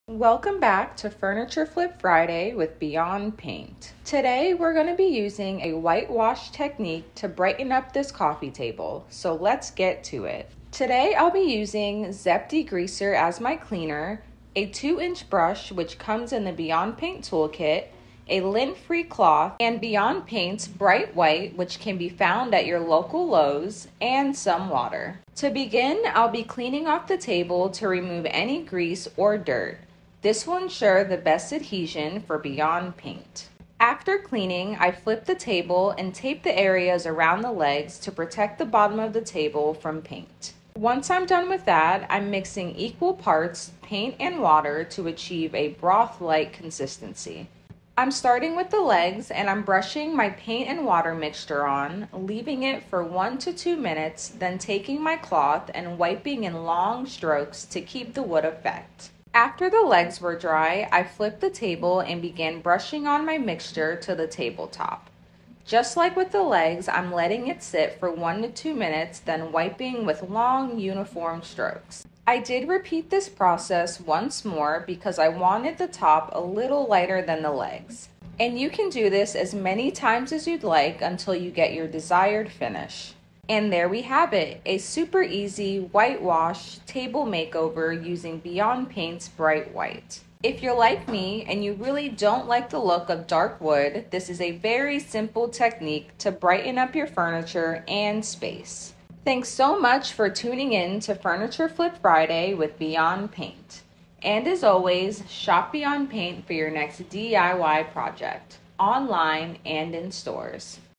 White Washing with Bright White
Watch this to see how we white wash a table using one of our top Designer Colors, Bright White 🤍✨